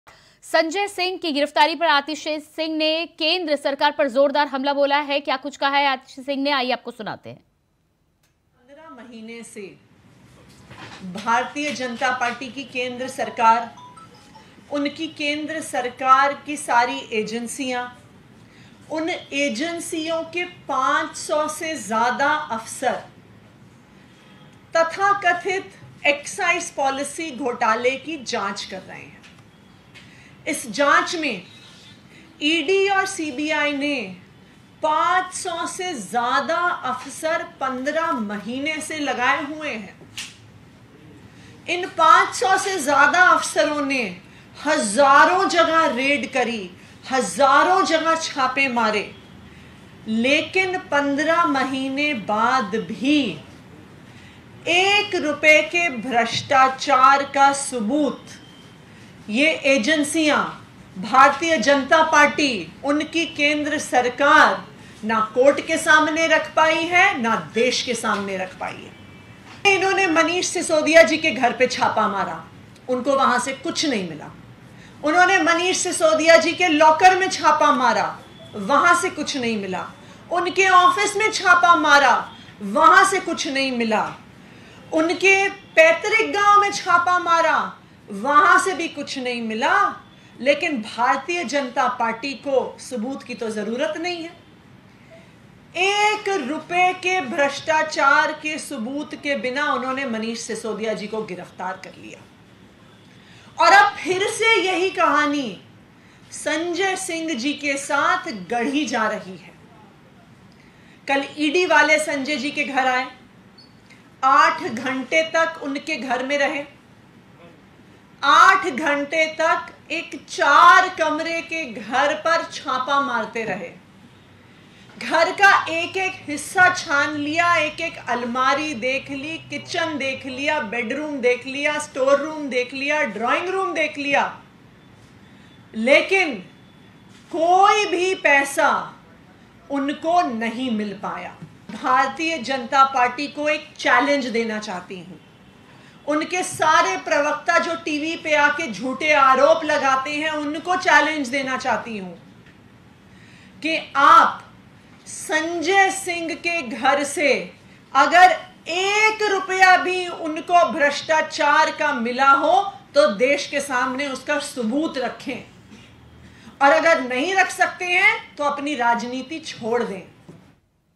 Sanjay Singh Arrested : AAP नेता Atishi ने Sanjay Singh की गिरफ्तारी पर बीजेपी को घेरा।
AAP नेता Atishi Marlena ने Sanjay Singh की गिरफ्तारी पर बीजेपी को घेरा संजय सिंह की गिरफ्तारी पर बोलते हुए दिल्ली सरकार में मंत्री आतिशी मार्लेना ने कहा कि, "आवाज दबाने के लिए उन्हें गिरफ्तार किया गया है." आतिशी बोलीं कि, "जो भी आवाज उठाएगा, उसके आवास पर छापेमारी की जाएगी."